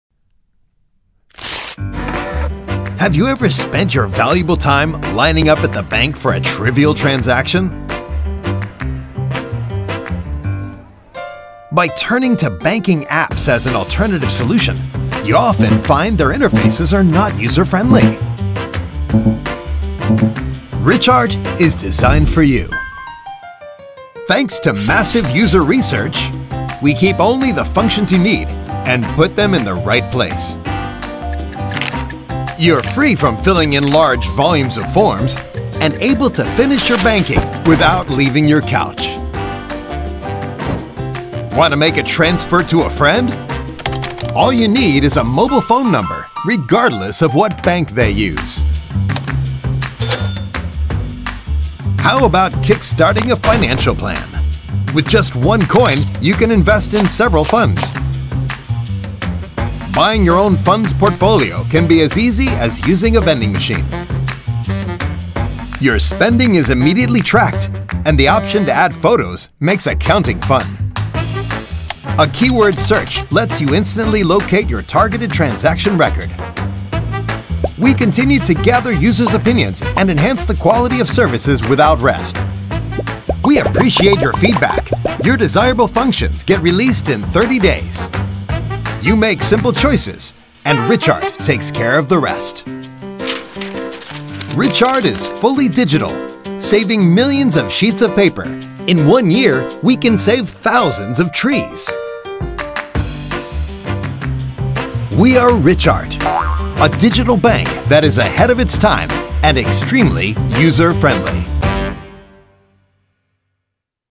2017 FinTech Days 台新銀行數位Richart 宣傳影片
2017年台灣金融研訓院舉辦 FinTech Days 盛會
以『金融科技前瞻高峰會暨創新創意資源大賞』為活動主軸，邀請國際大師來台分享，結合科技監理、人工智慧、區塊鏈、虛擬實境、大數據技術等相關主題活動，提供參加的金融業者、資訊廠商及民眾等產官學參考合作以及學習交流的機會。